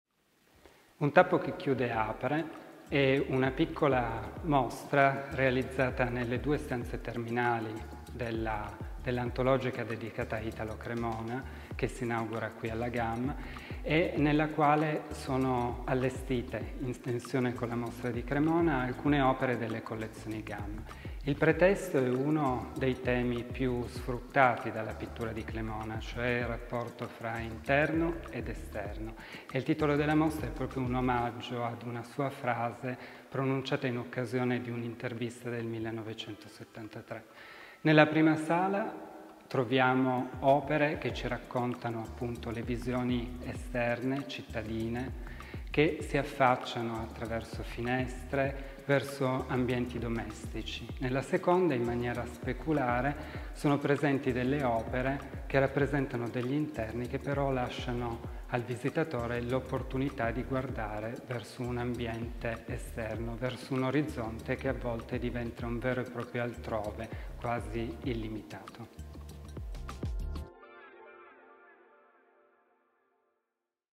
ITALO CREMONA. Tutto il resto è profonda notte - FOCUS 9
La GAM di Torino dedica all’opera e all’universo creativo di Italo Cremona una mostra antologica che ripercorre l’intera produzione dell’artista.
La mostra, a cura di Giorgina Bertolino, Daniela Ferrari, Elena Volpato, è frutto della collaborazione tra la GAM di Torino e il Mart di Rovereto, dove la mostra si trasferirà il prossimo autunno.